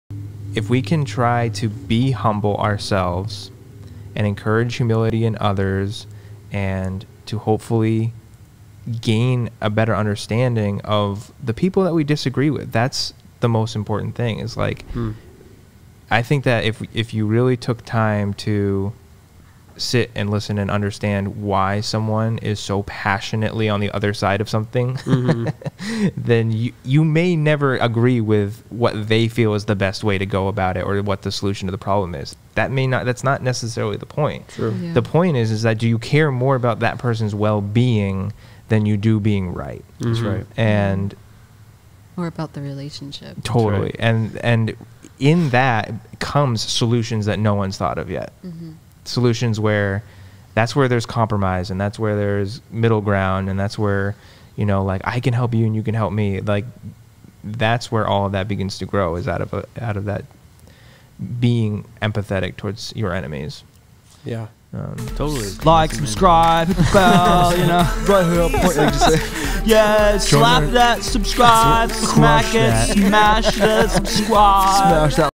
Society in 2020: Why should we empathize with our enemies?
In episode 5 of the Votary Podcast entitled Meaningful work in 2020, Cody, Jed, Jamin, and Graci talk about what our roles are as creatives in a divided society.